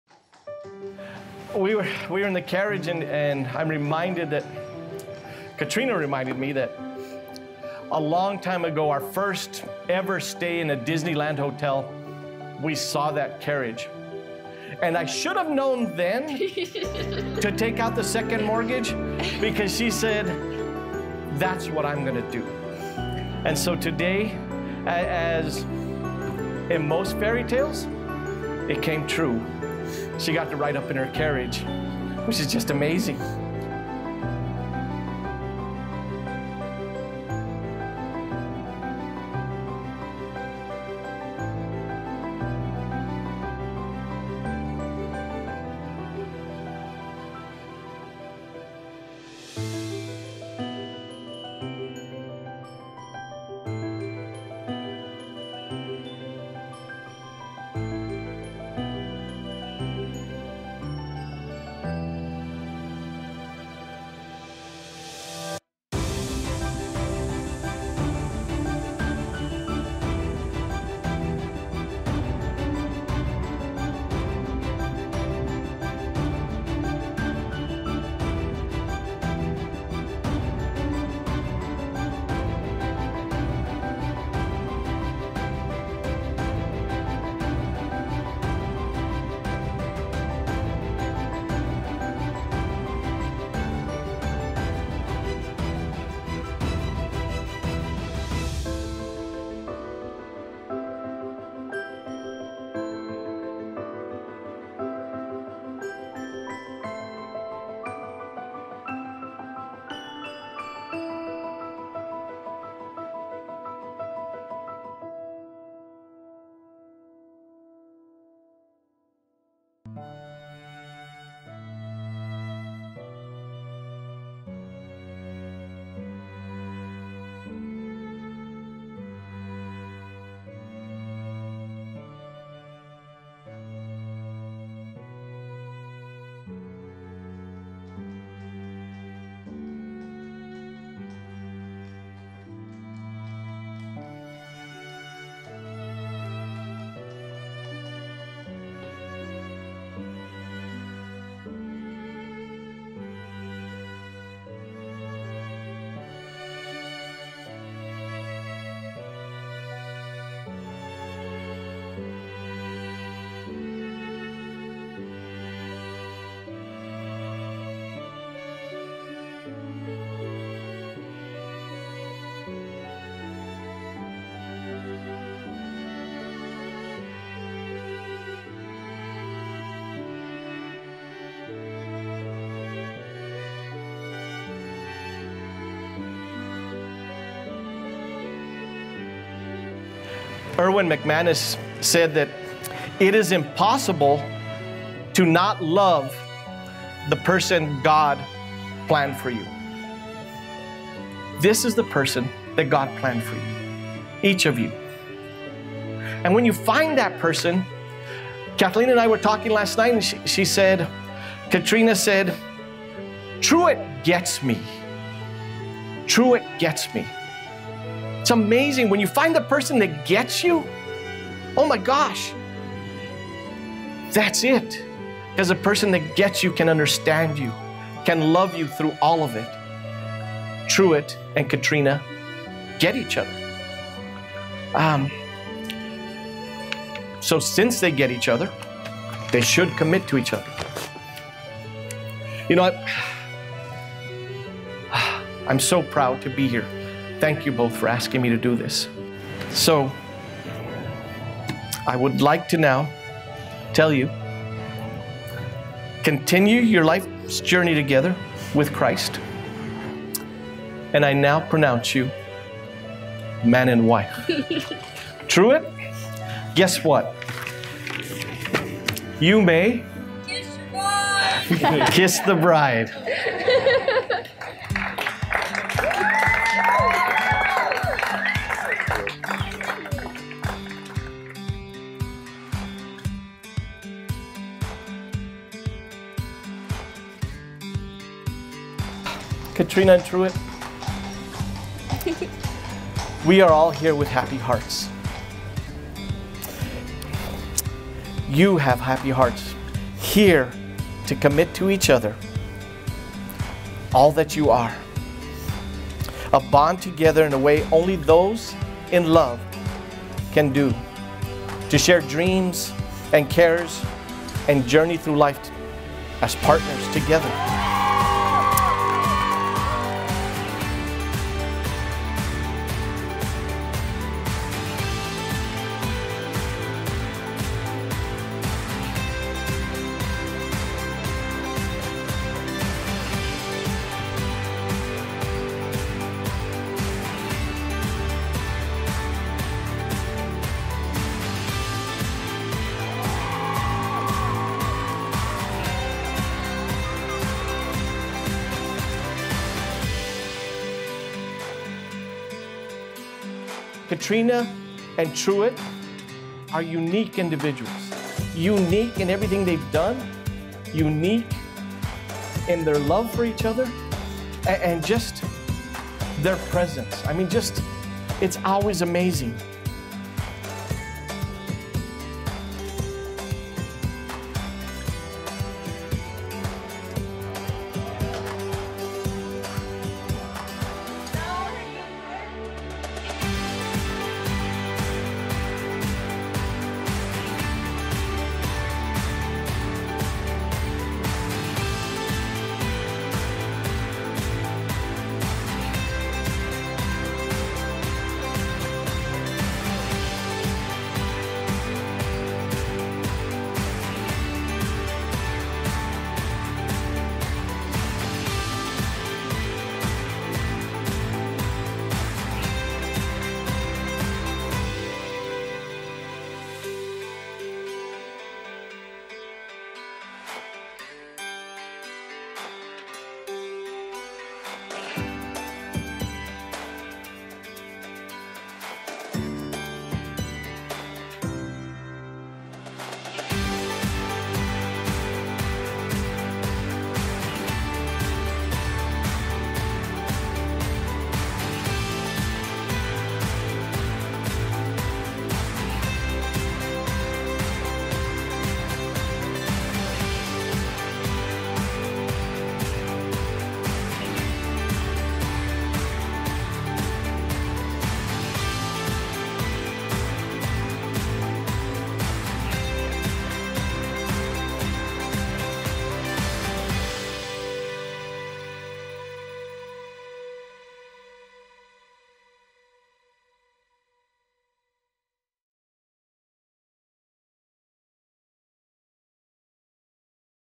Disney Wedding at Disneyland Hotel | Fairytale Wedding with Cinderella Carriage
Katryna and Truett have the wedding of their dreams at Disneyland Hotel in Anaheim, CA. Katryna told her dad when she was only 13 years old that someday she would get married at Disneyland as ride in the Cinderella Carriage with her groom. Katryna and Truett did just that and more at their beautiful Disney wedding with Mickey dropping in and greeting all her guests during the reception.
Wedding Video/ Wedding Film by: Jesse Madison Photography
Photography: Jesse Madison Photography
Dress: Vera Wang
Venue: Disneyland Hotel, Anaheim, CA
Wedding Coordinator: Hanna Dodd with Disney's Fairy Tale Weddings & Honeymoons